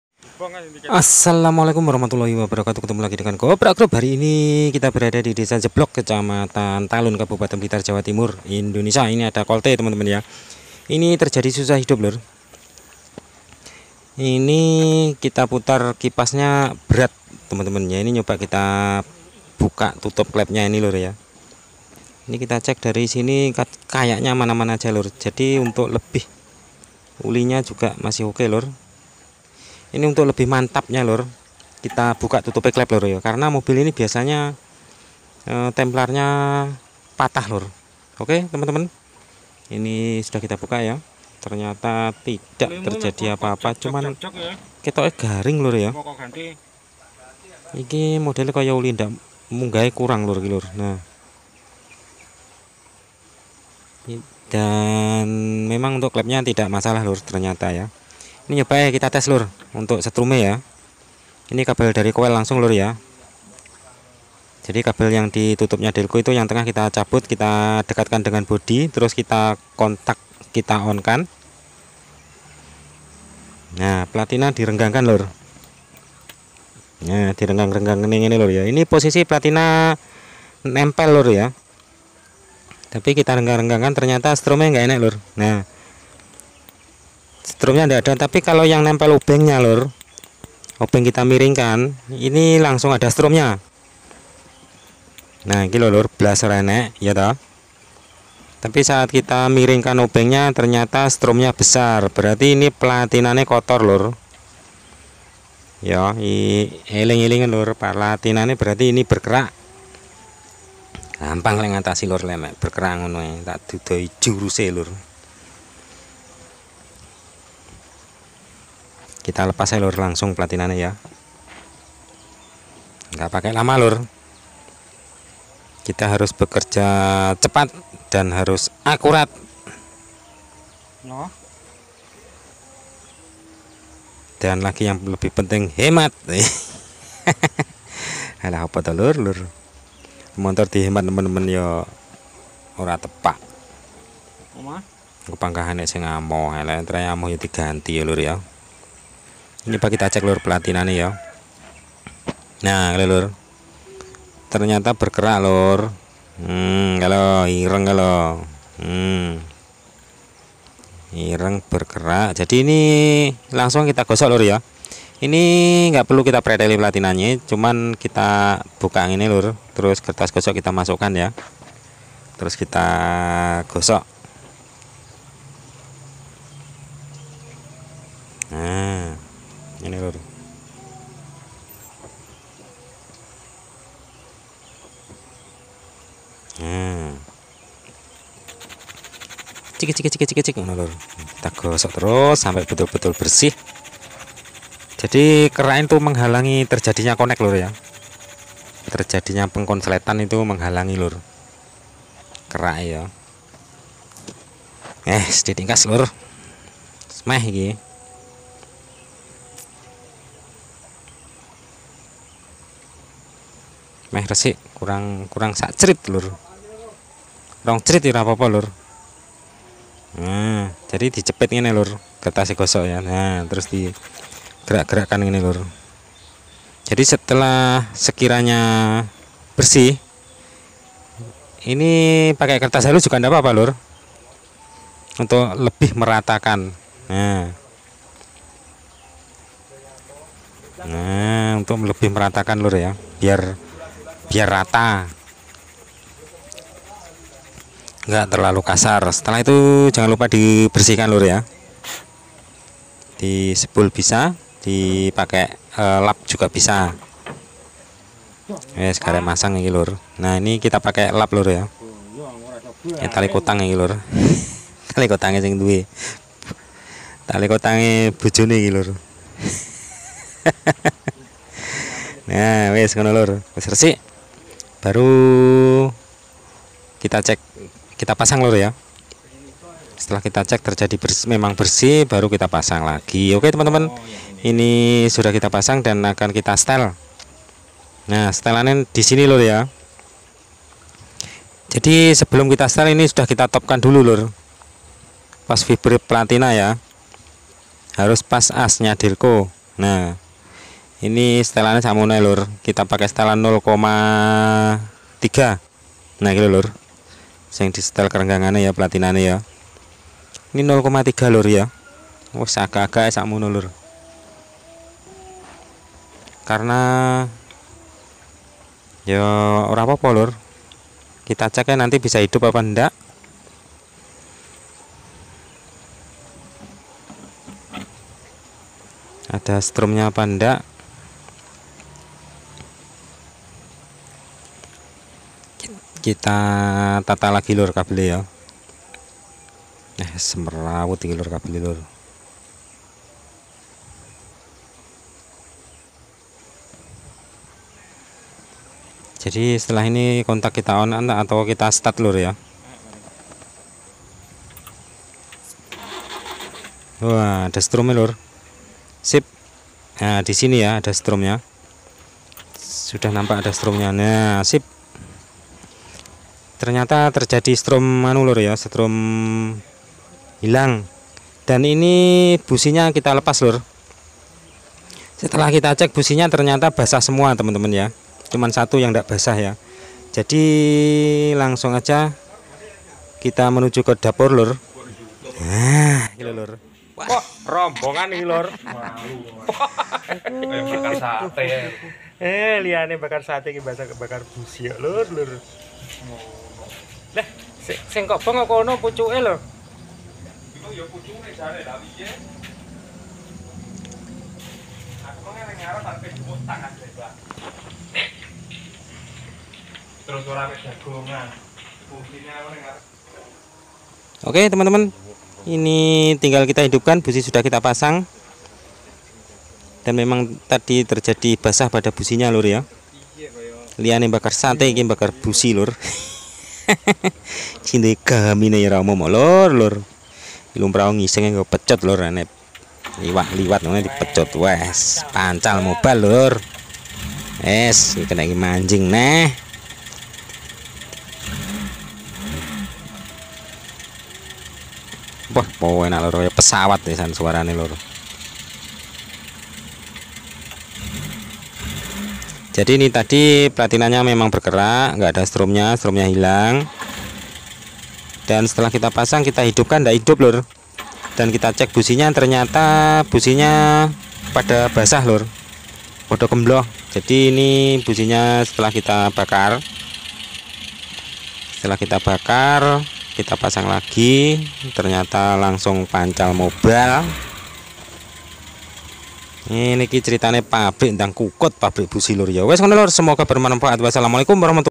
CARA HEMAT mengatasi MESIN SUSAH HIDUP
inilah penyebab mesin susah hidup